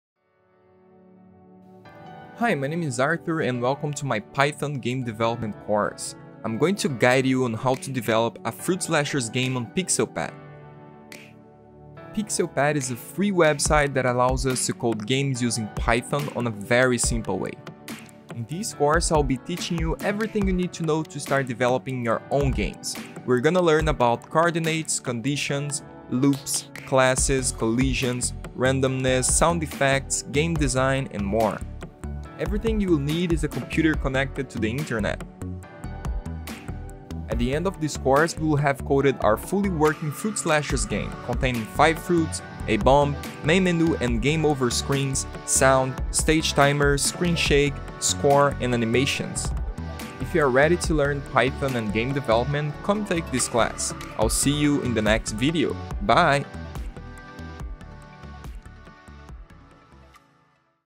PixelPAD Tutorial: Fruit Slashers #0 - Introduction
Learn Python by creating a Fruit Ninja like game on PixelPAD

Are you ready to learn Python and create your own games? In this course we are going to create a Fruit Ninja like game on PixelPAD, a web-based IDE. Everything you will need is a computer connected to the internet.

At the end of this course we will have coded our fully working Fruit Slashers game, containing 5 fruits, a bomb, main menu and game over screens, sound, stage timer, screen shake, score, and animations.

This course is going to teach you everything you need to start coding your own games:
- Programming Logic
- X & Y Coordinates
- Object Oriented Programming
- Conditions
- Loops
- Randomness
- Coding Best Practices
- Game Design